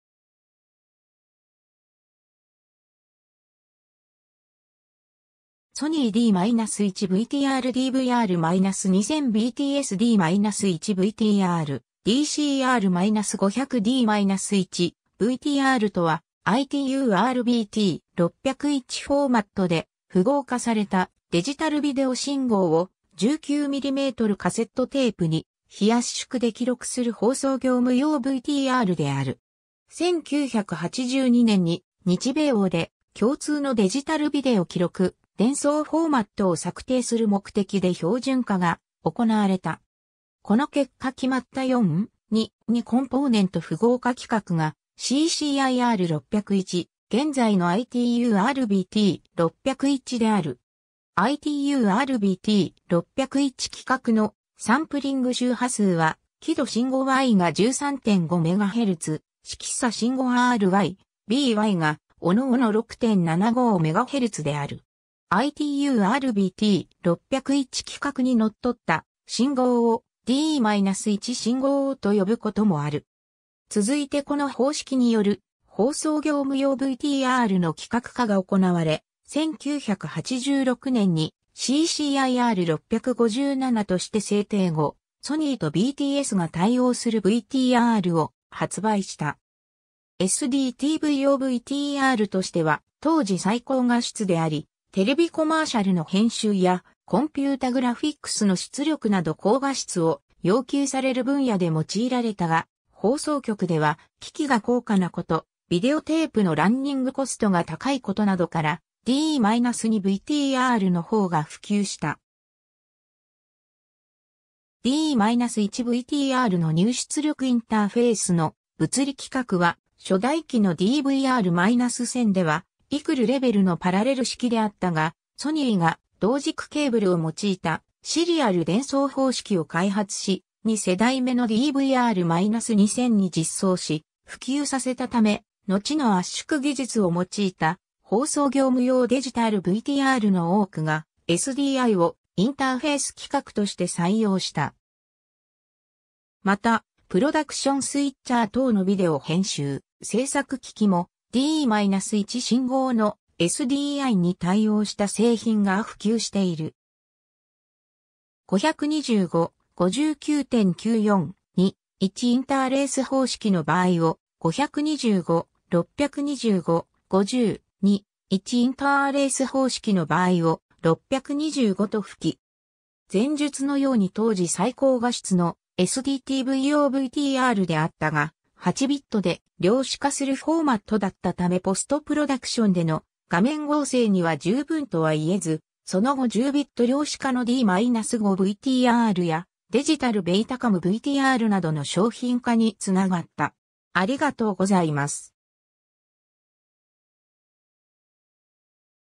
D1-VTR
Sony D-1 VTR DVR-2000 BTS D-1 VTR DCR-500 D-1 VTRとはITU-R BT.601フォーマット（4:2:2コンポーネント方式）で符号化されたデジタルビデオ信号を、19mm（約3/4インチ）カセットテープに非圧縮で記録する放送業務用VTRである。
 1982年に日米欧で共通のデジタルビデオ記録・伝送フォーマットを策定する目的で標準化が行われた。
この結果決まった4:2:2コンポーネント符号化規格がCCIR 601、現在のITU-R BT.601である。
ITU-R BT.601規格のサンプリング（標本化）周波数は、輝度信号Yが13.5MHz、色差信号R-Y, B-Yが各々6.75MHzである。
ITU-R BT.601規格に則った信号（D-1 VTRの入出力インターフェース信号）を「D-1信号」と呼ぶこともある。
 続いてこの方式による放送業務用VTRの規格化が行われ、1986年にCCIR 657として制定後、ソニーとBTS（ボッシュとフィリップスとの合弁放送機器メーカー、その後フランスのThomsonに買収され現在はGrass Valleyが継承）が対応するVTRを発売した。
 SDTV用VTRとしては当時最高画質であり、テレビコマーシャルの編集やコンピュータグラフィックスの出力など高画質を要求される分野で用いられたが、放送局では機器が高価なこと（VTRだけでなく編集設備もコンポーネント信号に対応させる必要がある、コンポジット映像信号用機器も残るので変換機器が必要など）、ビデオテープのランニングコストが高いことなどからD-2 VTRの方が普及した。
 D-1 VTRの入出力インターフェースの物理規格は初代機のDVR-1000ではECLレベルのパラレル式（ITU-R BT.656）であったが、ソニーが同軸ケーブルを用いたシリアル伝送方式を開発し、2世代目のDVR-2000に実装し普及させた（SMPTE 259Mとして規格化。SDIと略される。）ため、のちの圧縮技術を用いた放送業務用デジタルVTRの多くがSDIをインターフェース規格として採用した。
また、プロダクションスイッチャー等のビデオ編集・制作機器も「D-1信号」の「SDI」に対応した製品が普及している。
 ※525/59.94/2：1インターレース方式の場合を“525”、625/50/2：1インターレース方式の場合を“625”と付記。
 前述の様に当時最高画質のSDTV用VTRであったが、8bitで量子化するフォーマットだったため ポストプロダクションでの画面合成（キーイングなど）には十分とは言えず、その後10bit量子化のD-5 VTRやDigital BETACAM VTRなどの商品化に繋がった。